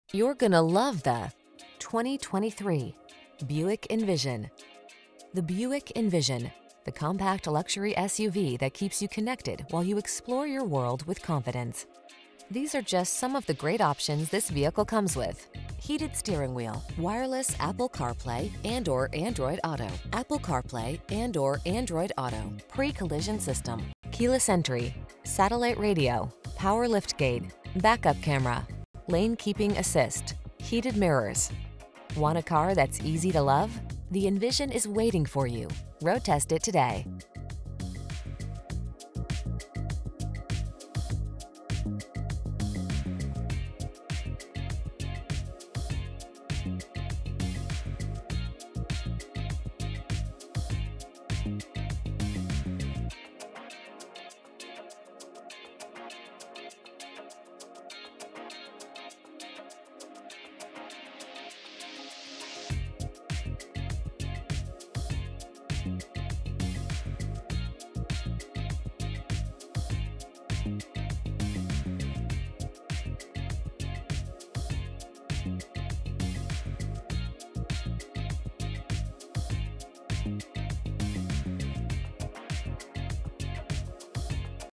2023 Buick Envision Preferred IL Naperville, Plainfield, Aurora, Oswego, Chicago
New 2023 Buick Envision Preferred available at Woody Buick located in 1585 W Ogden Ave, Naperville, IL, 60540. Servicing the  area.

You're gonna love the  2023  Buick Envision. The Buick Envision: The compact luxury SUV that keeps you connected while you explore your world with confidence. These are just some of the great options this vehicle comes with: Heated Steering Wheel
, Wireless Apple Carplay®/Android Auto®, Apple Carplay®/Android Auto®, Pre-Collision System, Keyless Entry, Satellite Radio, Power Liftgate, Back-Up Camera, Lane Keeping Assist, Heated Mirrors. Want a car that's easy to love? The Envision is waiting for you! Roadtest it today.